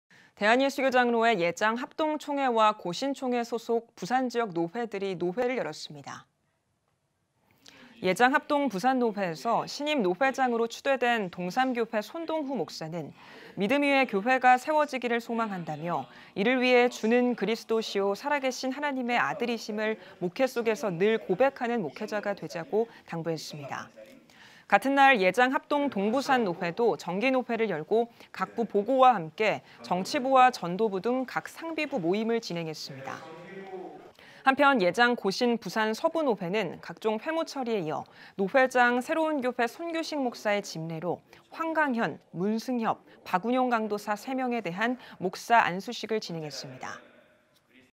부산지역 예장합동 및 고신 정기노회 (부산, 노경모) l CTS뉴스
대한예수교장로회 예장합동총회와 고신총회 소속 부산지역 노회들이 일제히 노회를 열었습니다.

예장 합동 부산노회에서 신임노회장으로 추대된 동삼교회 손동후 목사는 “믿음 위에 교회가 세워지기를 소망한다”며 “이를 위해 주는 그리스도시요 살아계신 하나님의 아들이심을 목회중심에 늘 고백하는 목회자가 되자“고 당부했습니다.

같은 날 예장 합동 동부산노회도 정기노회를 열고 각부 보고와 함께 정치부, 전도부 등 각 상비부 모임을 진행했습니다.

한편, 예장 고신 부산서부노회는 각종 회무처리에 이어 노회장 새로운교회 손규식 목사의 집례로 황강현, 문승협, 박운용 강도사 3명에 대한 목사안수식을 진행했습니다.